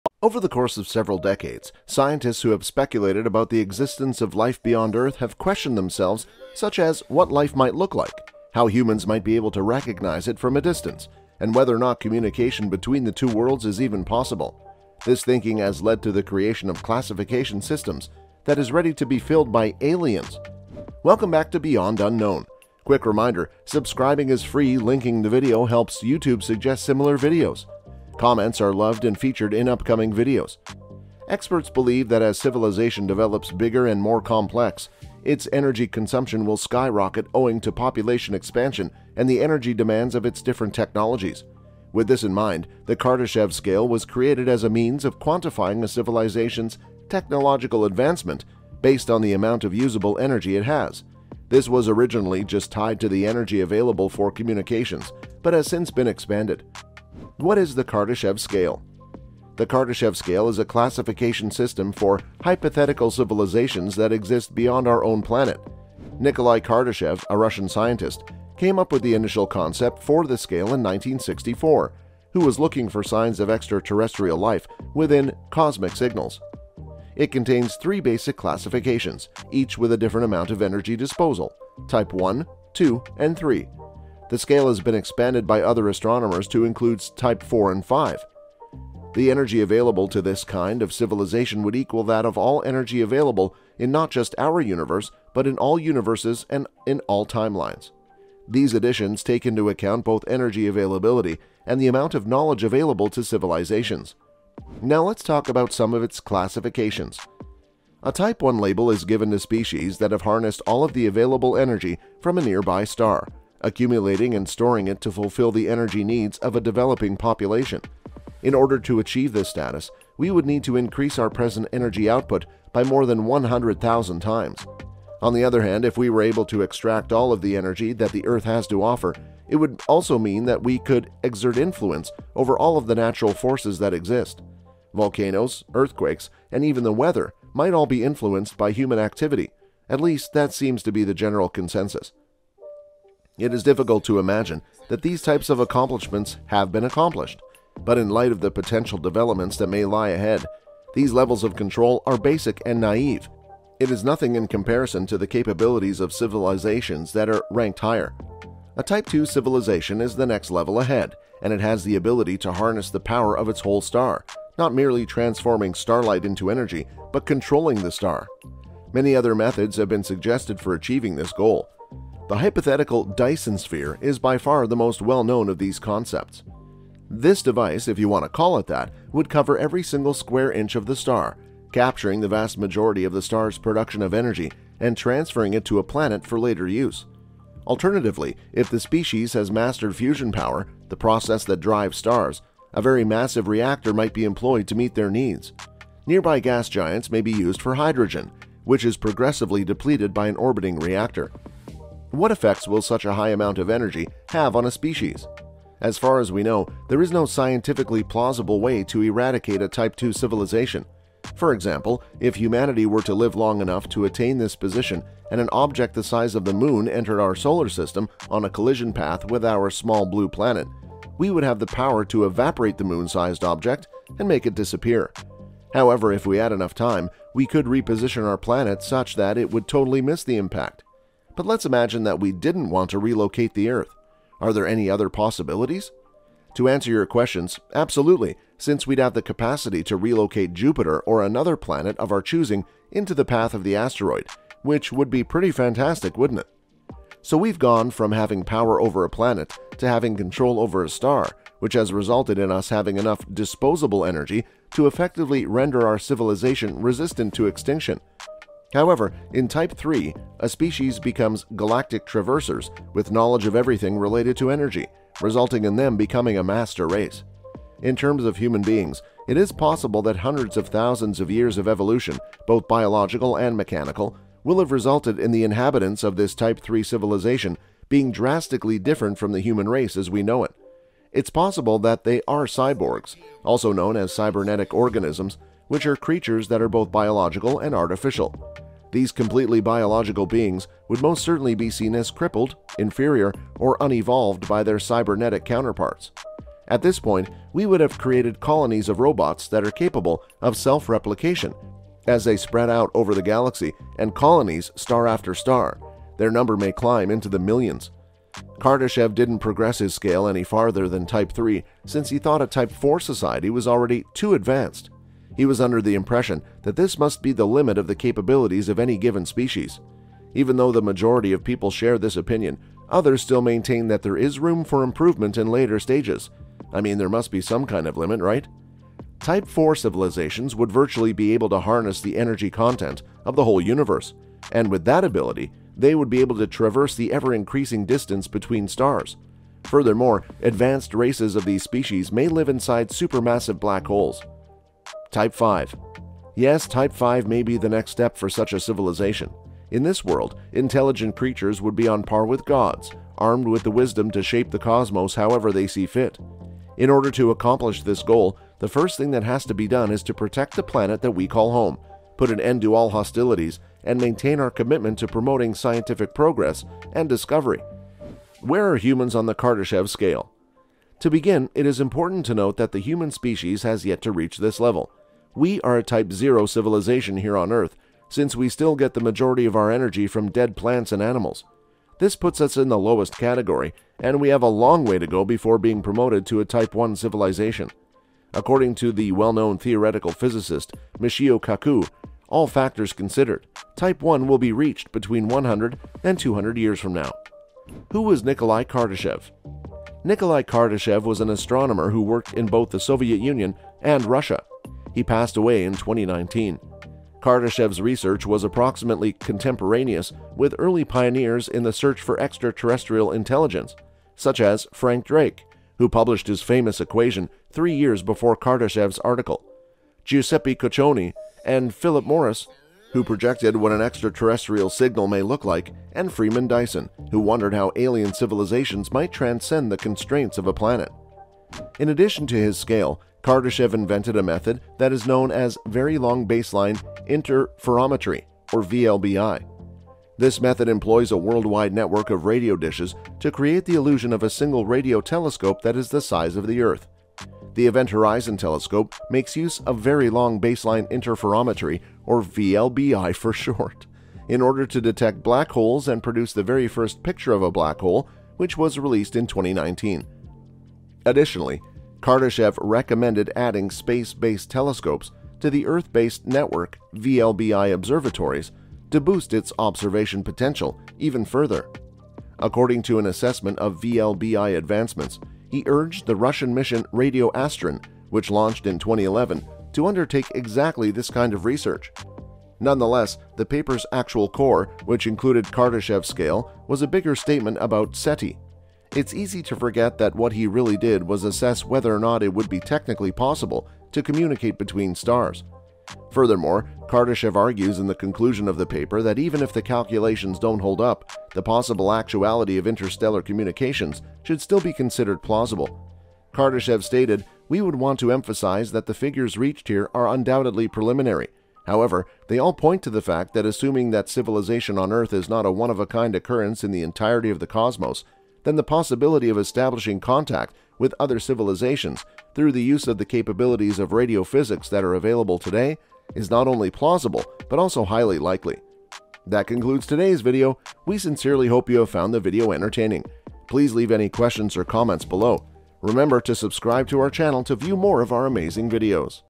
NASA Discovered A Possible Evidence Of Powerful Civilizations
Over the course of several decades, scientists who have speculated about the existence of life beyond Earth have questioned themselves such as what life might look like, how humans might be able to recognize it from a distance, and whether or not communication between the two worlds is even possible. This thinking has led to the creation of classification systems that is ready to be filled by aliens.

Welcome back to Beyond Unknown. Comments are loved and featured in upcoming videos

Experts believe that as civilization develops bigger and more complex, its energy consumption will skyrocket owing to population expansion and the energy demands of its different technologies. With this in mind, the Kardashev scale was created as a means of quantifying a civilization's technological advancement based on the amount of useable energy it has (this was originally just tied to the energy available for communications, but has since been expanded)

What is the Kardashev scale?

The Kardashev scale is a classification system for hypothetical civilizations that exist beyond our own planet. Nikolai Kardashev, a Russian scientist, came up with the initial concept for the scale in 1964. (Who was looking for signs of extraterrestrial life within cosmic signals). It contains three basic classifications, each with a different amount of energy disposal: Type I, Type II, and Type III.

The scale has been expanded by other astronomers to include Type IV and Type V. (the energy available to this kind of civilization would equal that of all energy available in not just our universe, but in all universes and in all timelines). These additions take into account both energy availability and the amount of knowledge available to civilizations.

Now let’s talk about some of its classifications

A Type I label is given to species that have harnessed all of the available energy from a nearby star, accumulating and storing it to fulfill the energy needs of a developing population. In order to achieve this status, we would need to increase our present energy output by more than 100,000 times.
On the other hand, if we were able to extract all of the energy that the Earth has to offer, it would also mean that we could exert influence over all of the natural forces that exist. Volcanoes, earthquakes, and even the weather might all be influenced by human activity. (At least, that seems to be the general consensus.)

It is difficult to imagine that these types of accomplishments have been accomplished, but in light of the potential developments that may lie ahead, these levels of control are basic and naive (it is nothing in comparison to the capabilities of civilizations that are ranked higher).
A Type II civilization is the next level ahead, and it has the ability to harness the power of its whole star (not merely transforming starlight into energy, but controlling the star). Many other methods have been suggested for achieving this goal. The hypothetical 'Dyson Sphere' is by far the most well-known of these concepts.

This device, if you want to call it that, would cover every single square inch of the star, capturing the vast majority of the star's production of energy and transferring it to a planet for later use. Alternatively, if the species has mastered fusion power (the process that drives stars), a very massive reactor might be employed to meet their needs. Nearby gas giants may be used for hydrogen, which is progressively depleted by an orbiting reactor.